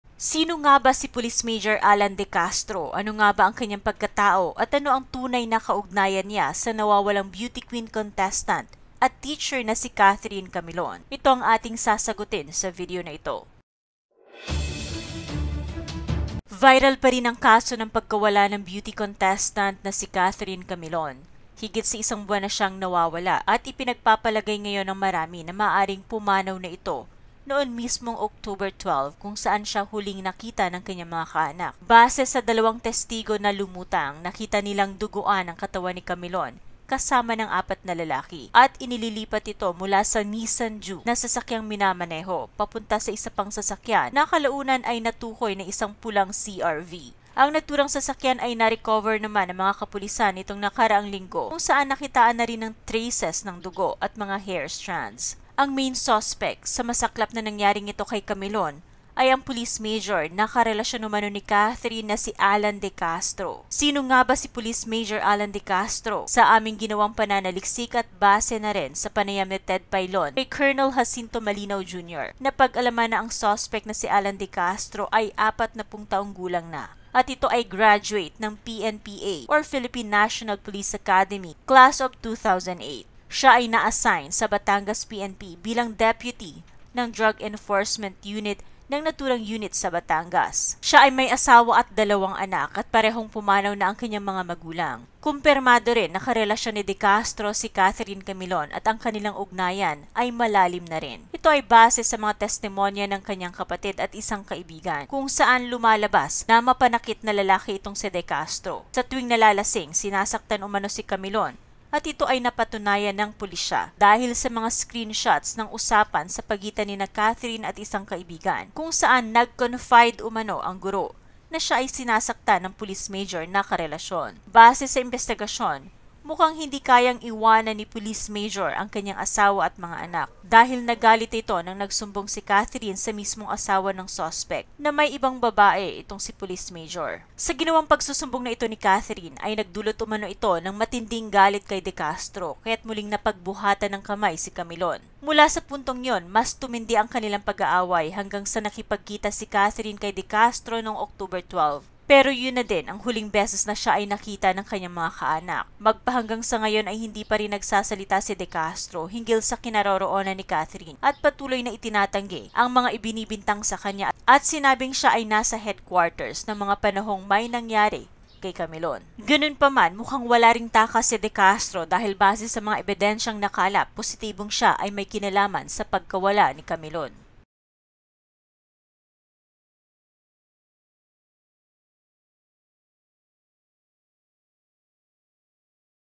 Tunay na PAGKATAO ni Police Major Allan De Castro ang Susppek sa Pagkawala ni Catherine Camilon
-sino nga ba si police major allan de castro? ano nga ba ang kanayng pagkatao at ano ang tunay na kauganyan niya sa nwawalang beauty queen contestant at teabher na si catherine camilon. ito ang ating ssagutin sa video na ito.